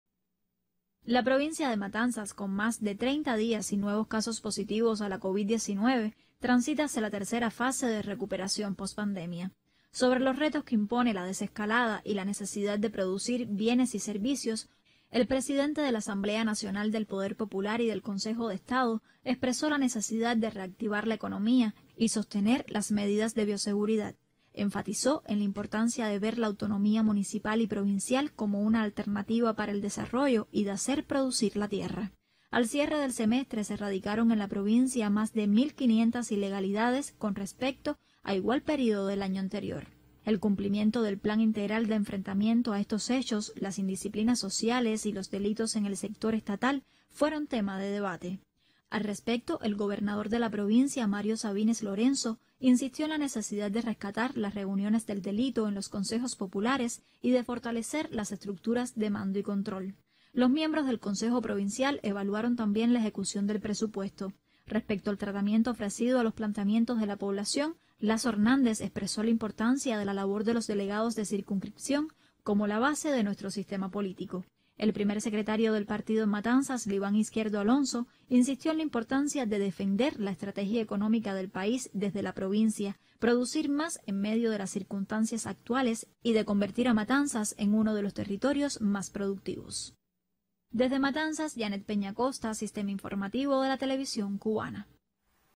Esteban Lazo participa en reunión del Consejo Provincial de Matanzas
El Miembro del Buró Político del Comité Central del Partido y Presidente de la AsambleaNacional del Poder Popular Esteban Lazo Hernández participó en la reunión del Consejo Provincial de Matanzas. En el encuentro analizaron asuntos asociados al ordenamiento y el desarrollo económico del territorio.